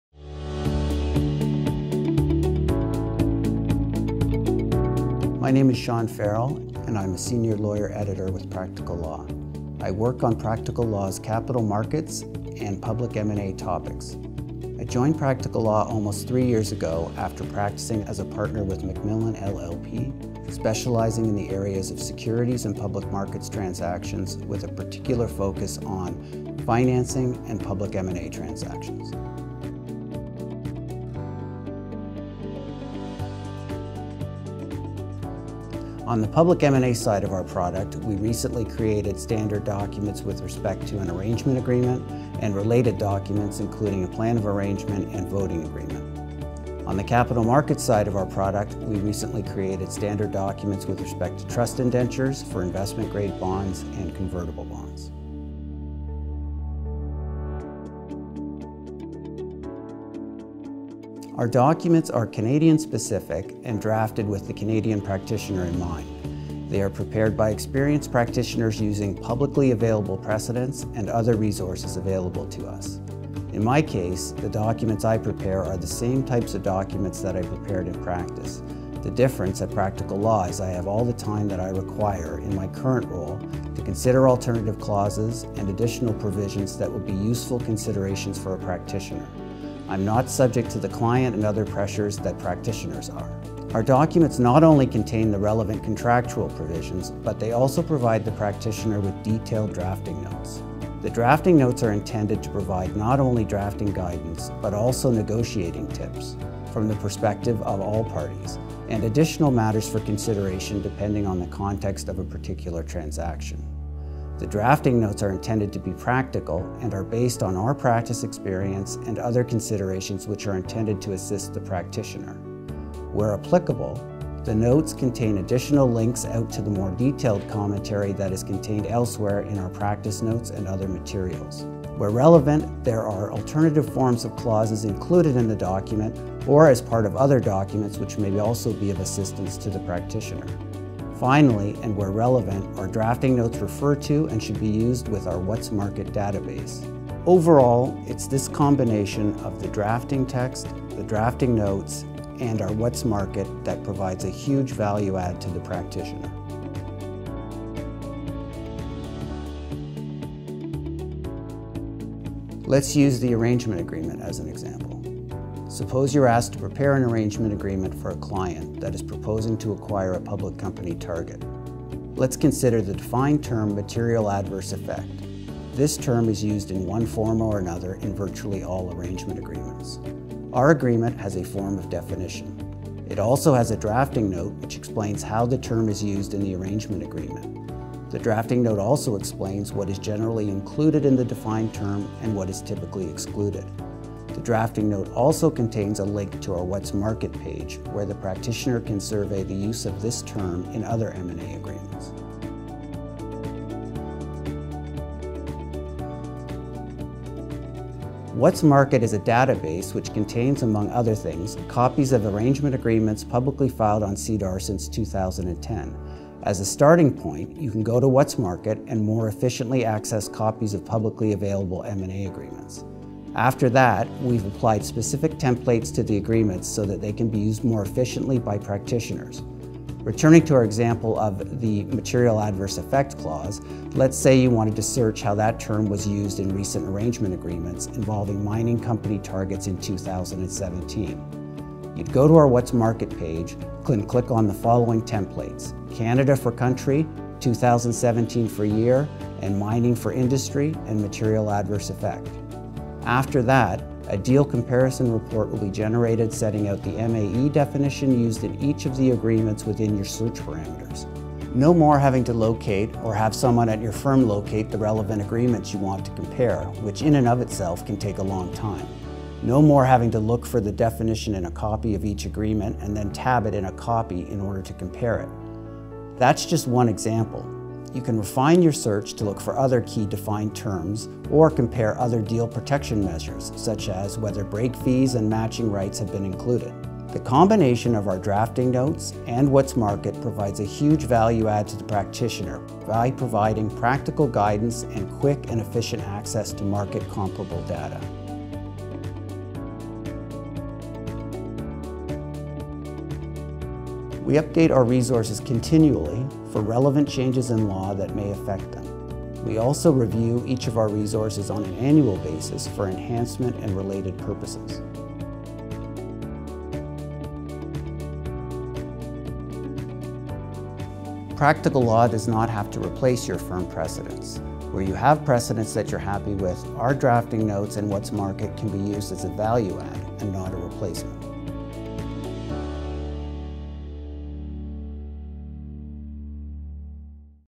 Practical Law Canada | Insights from Sean Farrell, Capital Markets & Securities Senior Lawyer-Editor
Sean is a senior lawyer-editor for Practical Law Canada’s Capital Markets & Securities service. He gives his views on why Practical Law Canada is unique and why it’s a tool that lawyers should consider.